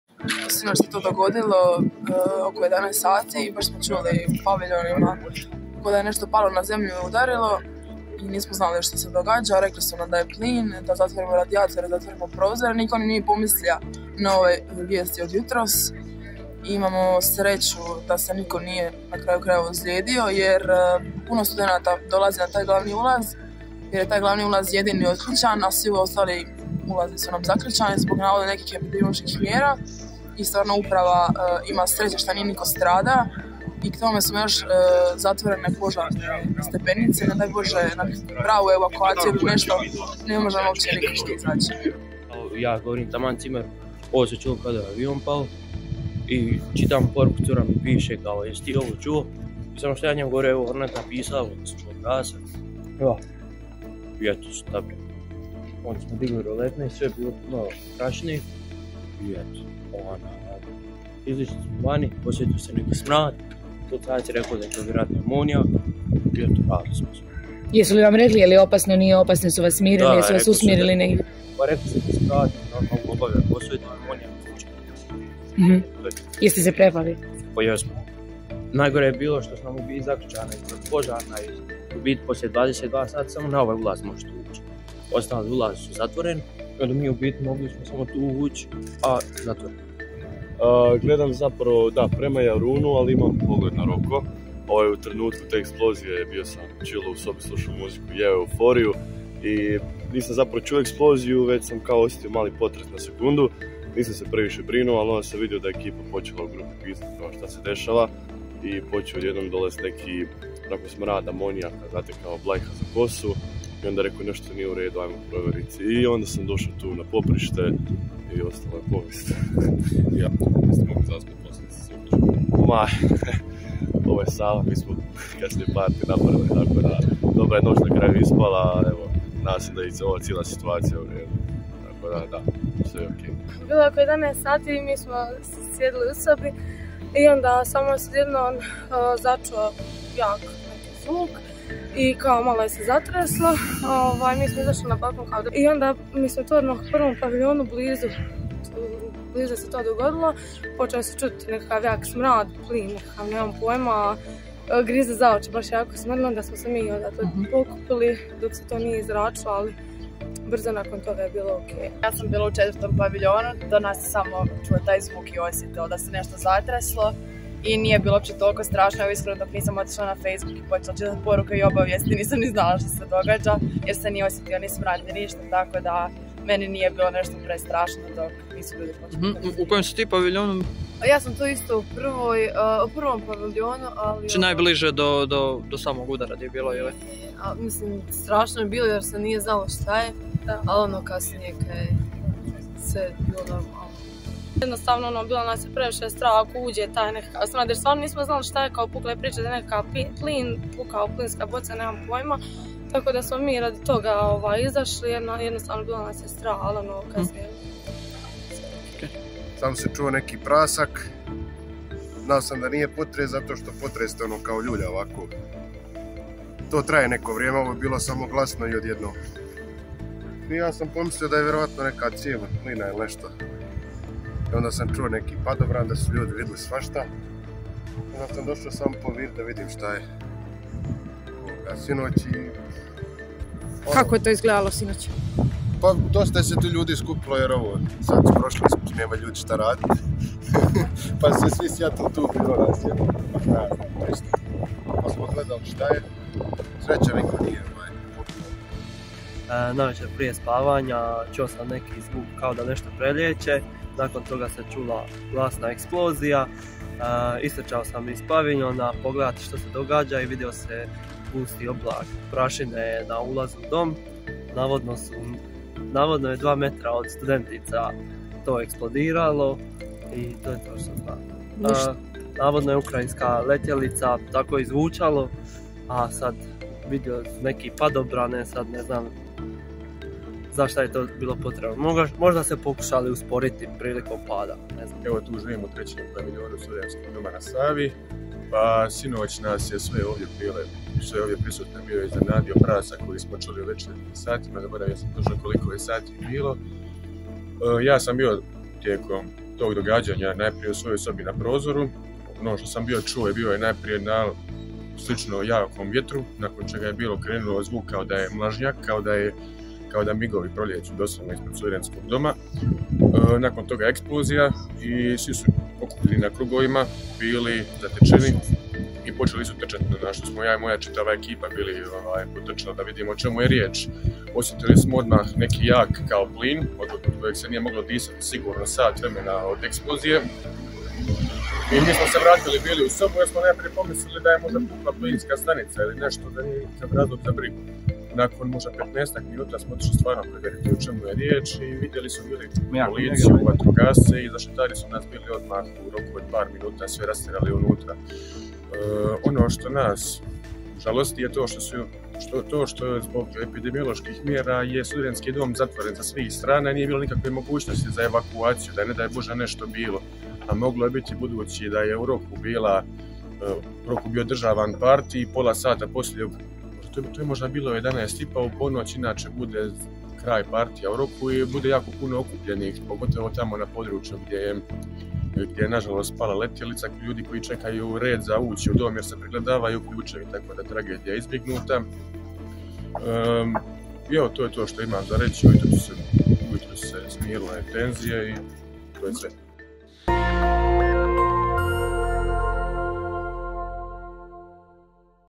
Studenti o eksploziji u Zagrebu: 'Vidjeli smo gusti oblak prašine. Sreća što nema poginulih'
Studente u domu Stjepan Radić kod Savskog mosta sinoć je uznemirila eksplozija u blizini doma.